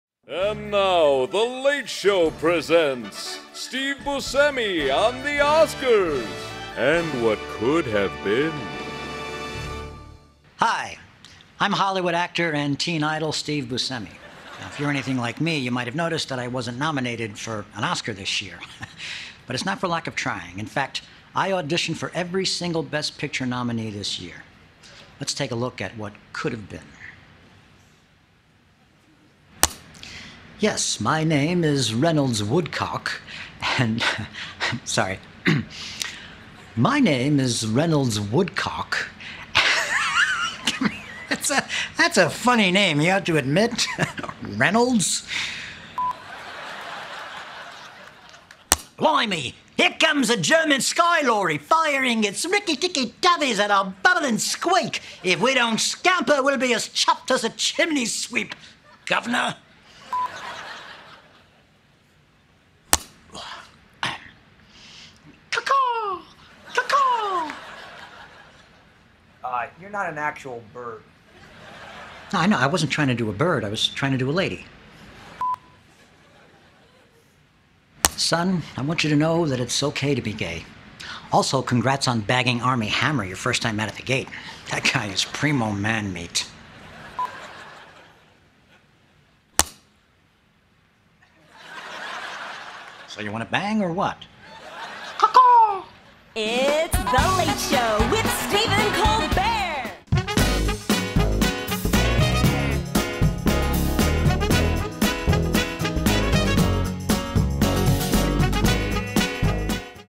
Steve Buscemi Auditioned For Every 'Best Picture' Nominee
Lady Bird, Get Out, The Darkest Hour... are all movies that did not feature Steve Buscemi.